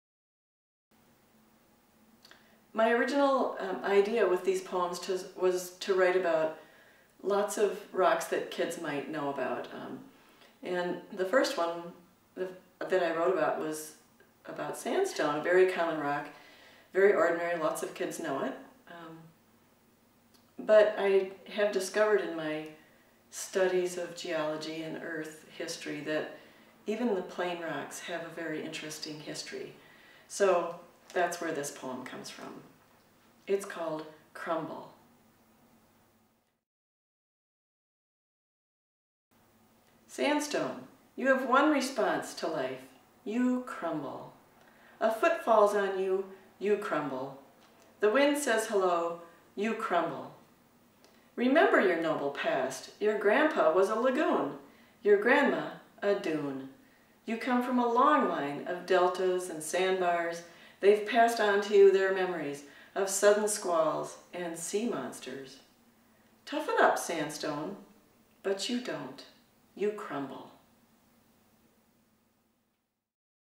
Crumble! by Lisa Westberg Peters
Poem is from Earthshake: Poems from the Ground Up by Lisa Westberg Peters. Video by Antonio Rodriguez.